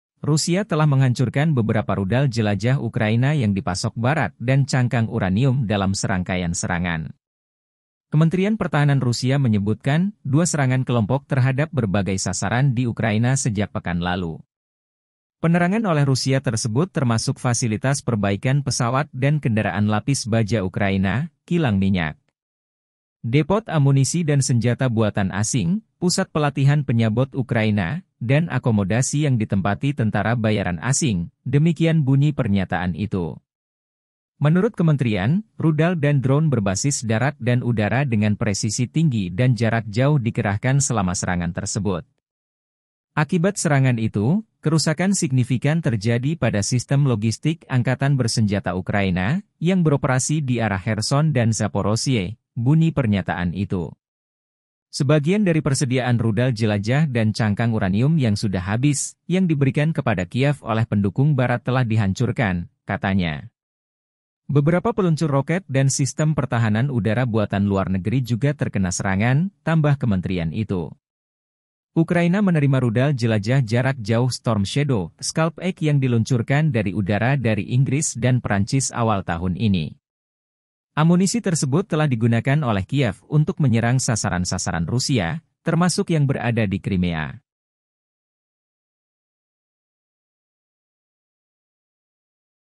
GERAK CEPAT RUSIA Hancurkan Gudang Senjata Ukraina yang Baru Saja Dipasok, Zelensky Rugi Besar?
TRIBUN-VIDEO.COM -- Rusia telah menghancurkan beberapa rudal jelajah Ukraina yang dipasok Barat dan cangkang uranium dalam serangkaian serangan.

Kementerian Pertahanan Rusia menyebutkan, 2 serangan kelompok terhadap berbagai sasaran di Ukraina sejak pekan lalu.

Penerangan oleh Rusia tersebut termasuk fasilitas perbaikan pesawat dan kendaraan lapis baja Ukraina, kilang minyak.

Artikel ini telah tayang di Tribunnews.com dengan judul Baru Dipasok, Cangkang Uranium dan Rudal Jelajah Ukraina Dihancurkan Rusia,